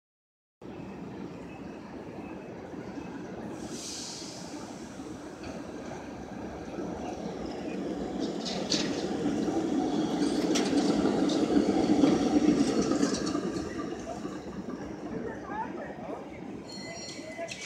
SF Muni 1947 St. Louis Car Co. PCC 1071
Here is a Not in Service PCC at Market & Main.

Motors: (4) General Electric 1220
Livery: Twin City Rapid Transit in Minneapolis-St. Paul, MN
From: New Jersey Transit in Newark, NJ
Division: Cameron Beach Yard